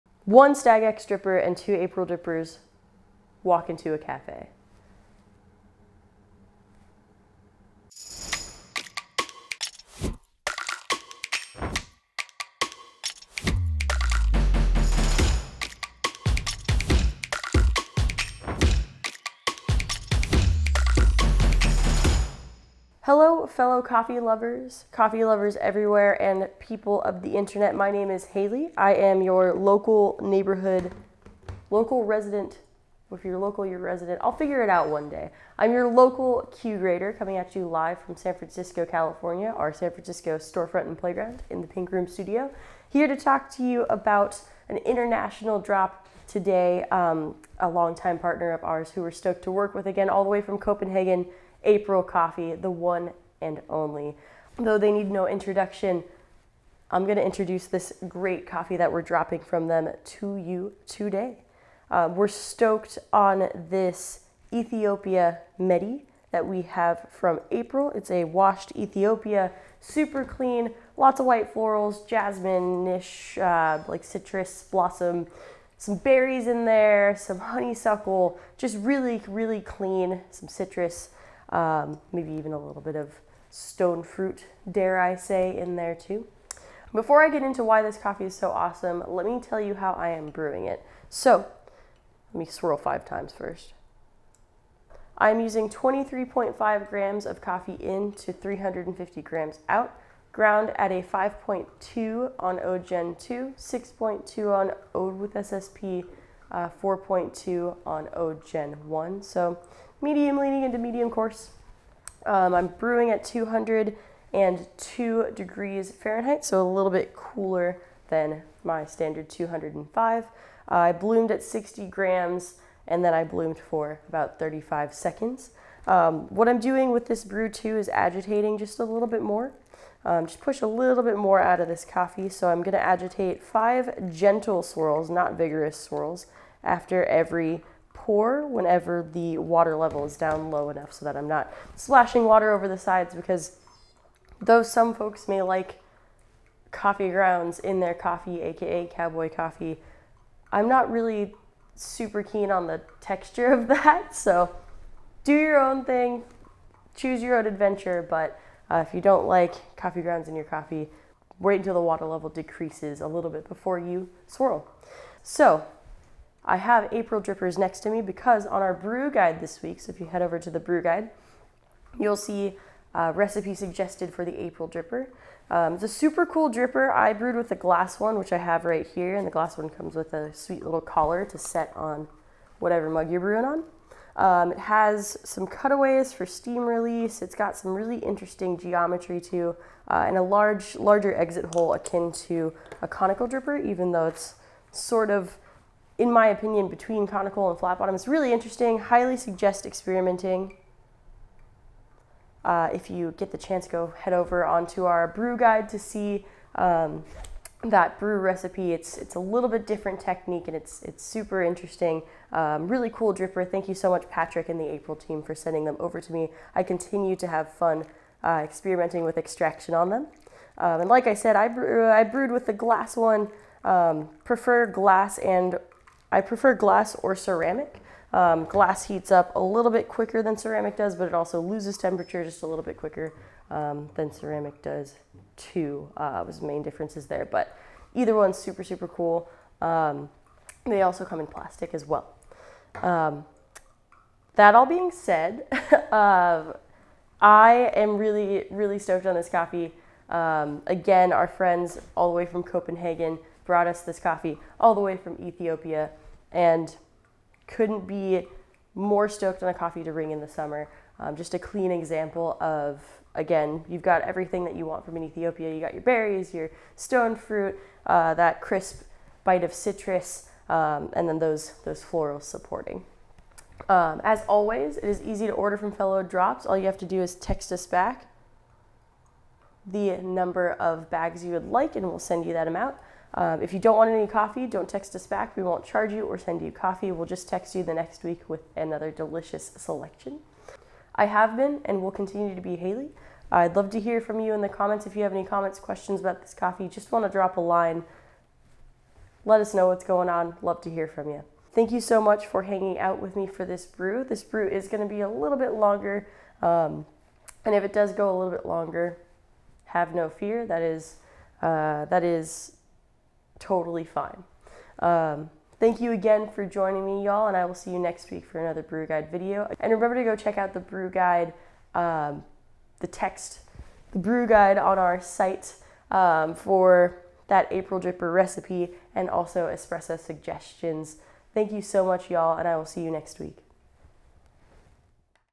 Fellow's Take on Ethiopia Meti from April | Brew Guide
We have a returning Drops-favorite roaster today. All the way from Copenhagen, Denmark: Ethiopia Meti by April! 🇩🇰

This is April’s first release from the new harvest out of Ethiopia, and it’s kicking off the season with a clean bouquet of sweet floral notes. We tasted honeysuckle, strawberry, honeydew melon, a clean pear acidity, and a silky tea-like body. Look out for the berry candy fragrance and the pop of crisp orange as it cools.

🌍 Origin: Hambela, Ethiopia
💦 Process: Washed
🔬 Varietal: Krume 74122
⛰️ Elevation: 1,800 MASL

This way to a brighter, less boring morning ⤵️

We're on Instagram!